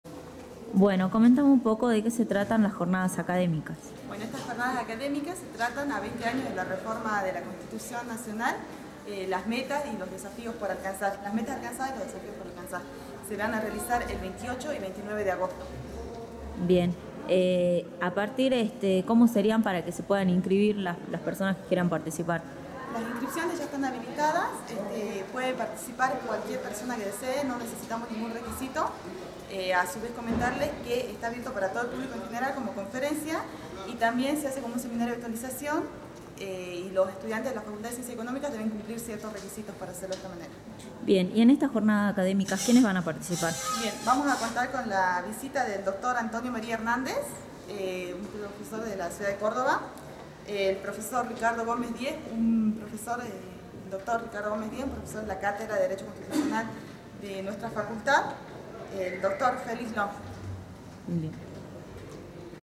Jornadas Académica en la UNSa
entrevista a Maria Villca Presidenta del centro de estudiantes de económica, sobre las jornada Académicas que se realizaran 28 y 29 de Agosto.